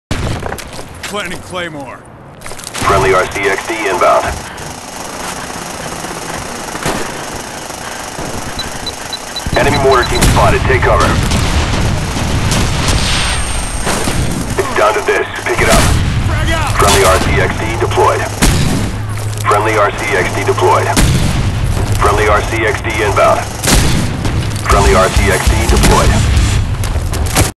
XSlevinn - RCXD Army
I use my stock piled 6 RCXD's to get some kills.